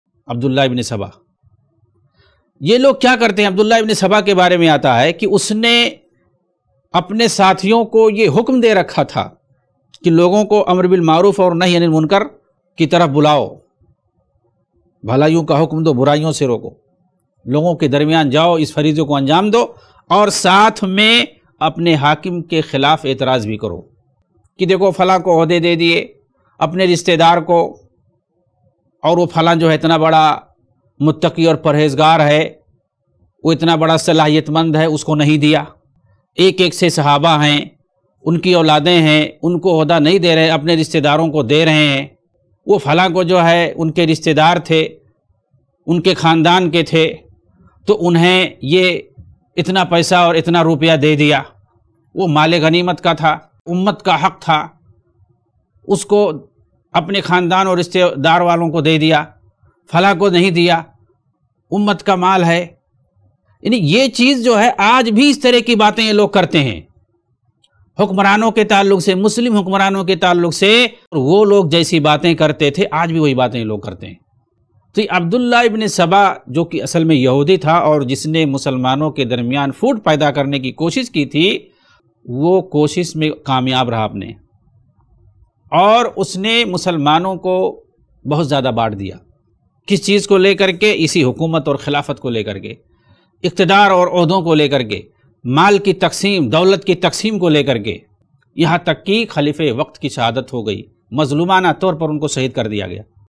Khawarijon Ko Abdullah ibne Saba Aise Bhadkata Tha Hakim E Muslim Ke Khilaaf by Dr. Ajmal Manzoor
Khawarijon Ko Abdullah ibne Saba Aise Bhadkata Tha Hakim E Muslim Ke Khilaaf by by Dr. Ajmal Manzoor Madani iPlus TV

UPI : donateilm@icici